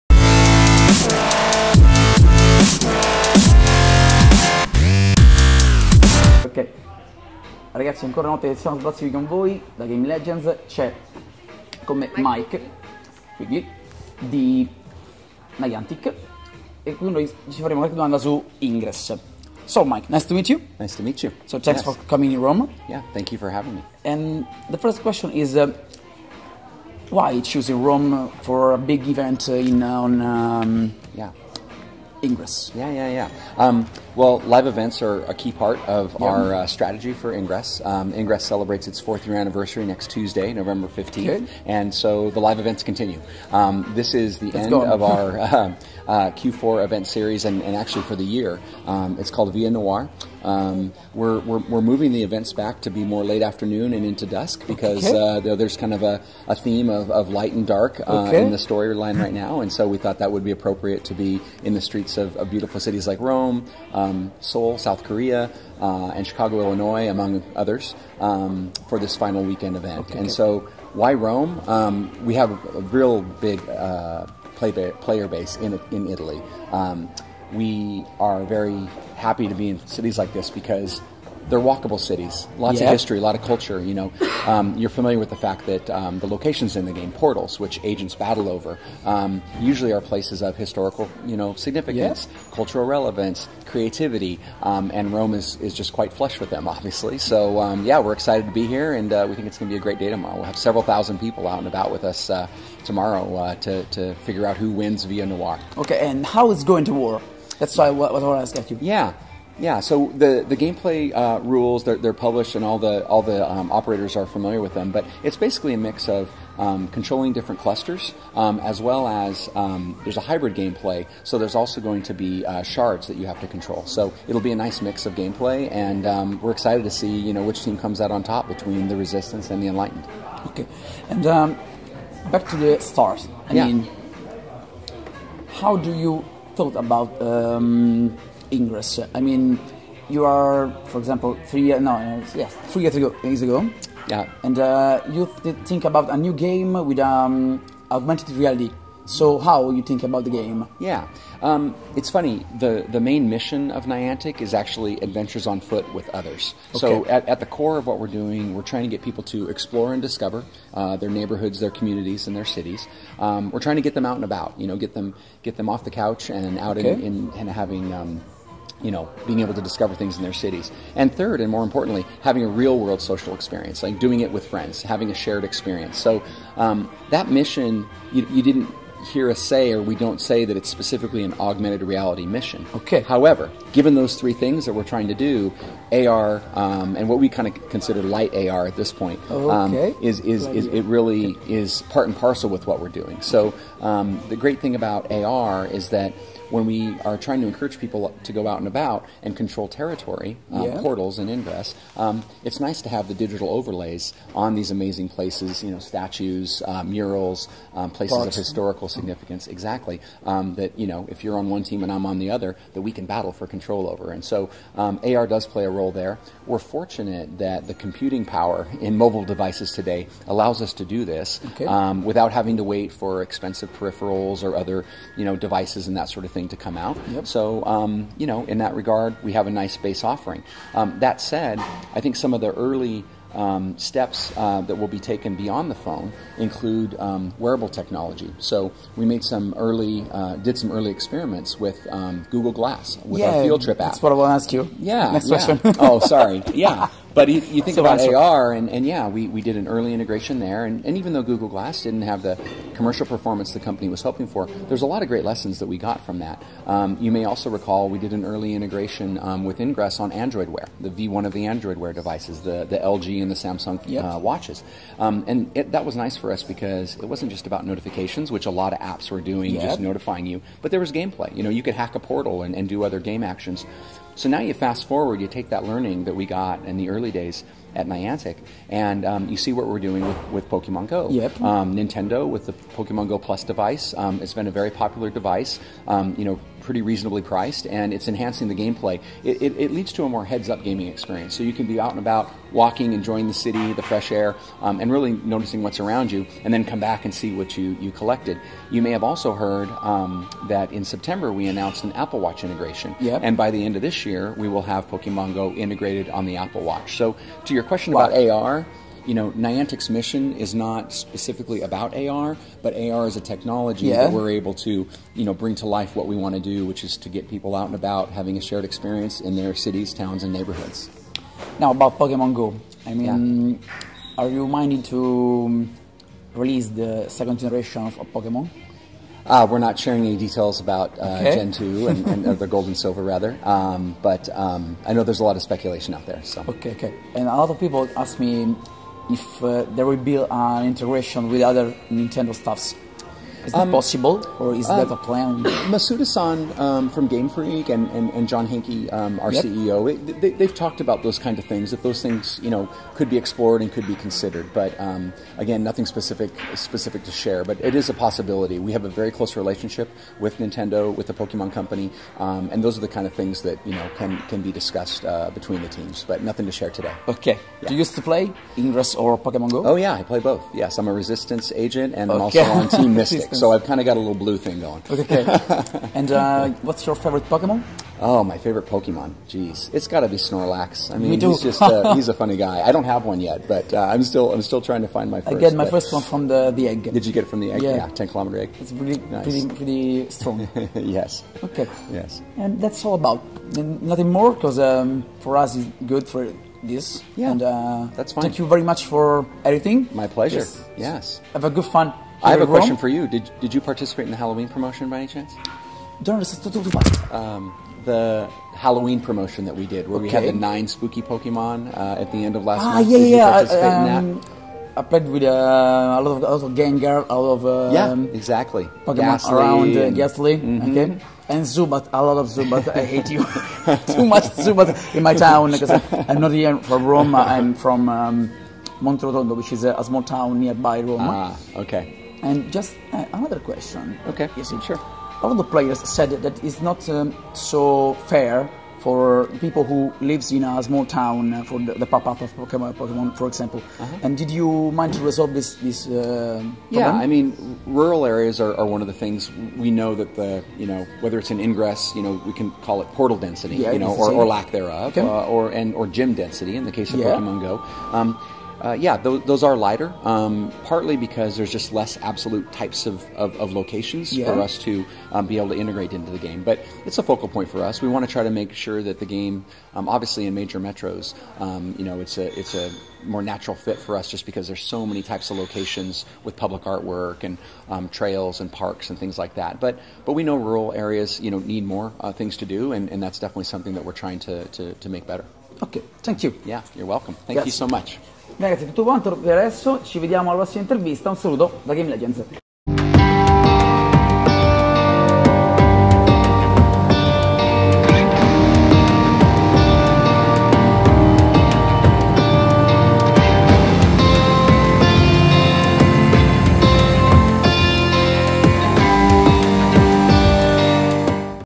Intervista a Mike Quigley di Niantic
Il nostro Tizio ha avuto la possibilità di intervistare Mike Quigley di Niantic, che ci ha parlato di Ingress, del suo successo, ma non solo! Scopriamo insieme cosa ha risposto alle nostre domande.

SEGUICI!!